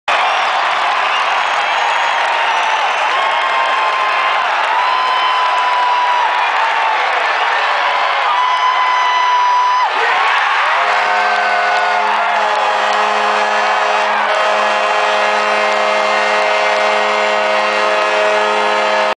Radim Vrbata shoot out goal
shoot out goal against detroit on dec 29